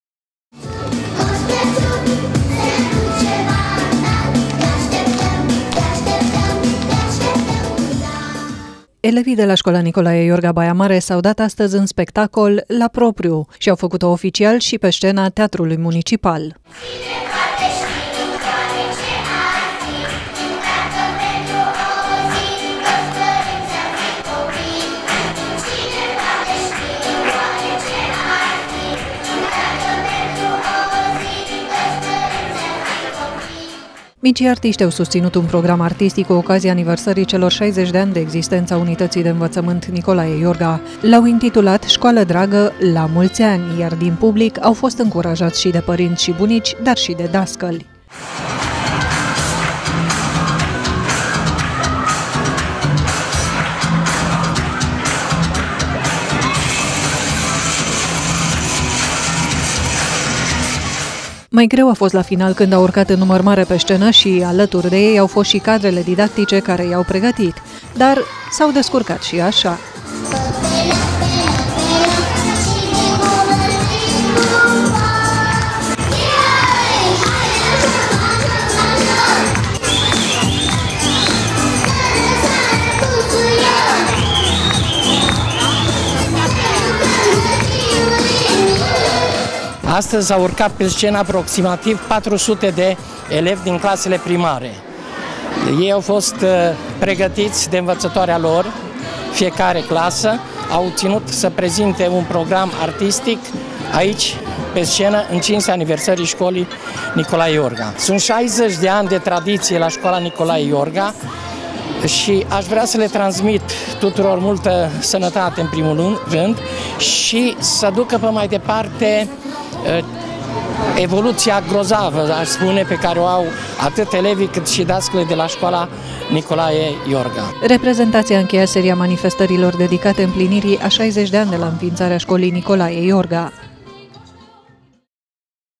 "Școală dragă, la mulți ani!"
Un spectacol, de lumini, sunet și culoare au oferit astăzi elevii din clasele primare ale Școlii Nicolae Iorga Baia Mare.
Micii artiști și-au susținut reprezentația pe scena Teatrului Muncipial Baia Mare, în fața dascălilor, a părinților și a bunicilor.